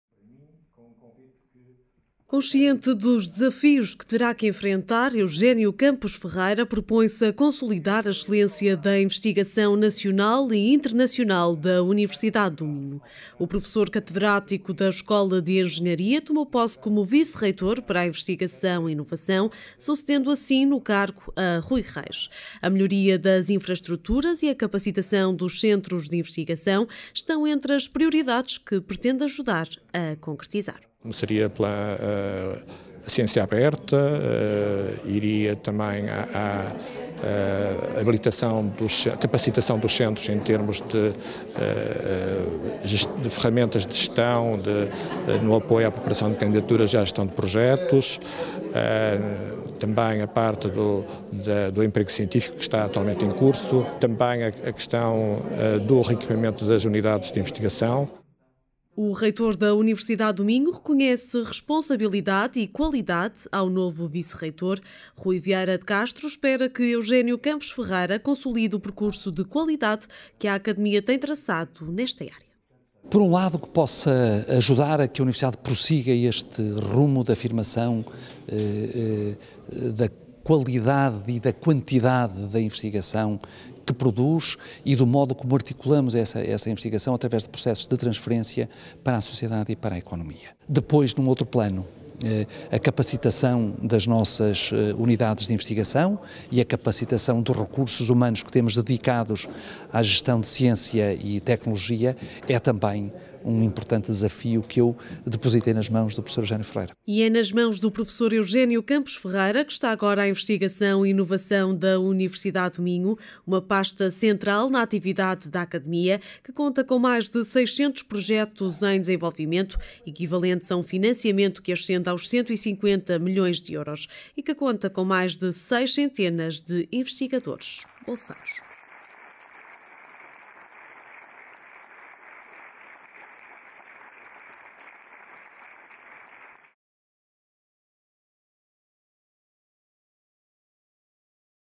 Eugénio Campos Ferreira é o novo vice-reitor da UMinho
Eugénio Campos Ferreira, professor catedrático da Escola de Engenharia, é docente do Departamento de Engenharia Biológica e diretor do Centro de Engenharia Biológica, foi empossado como vice-reitor da UMinho para as áreas da Investigação e Inovação. Sucede no cargo a Rui L. Reis.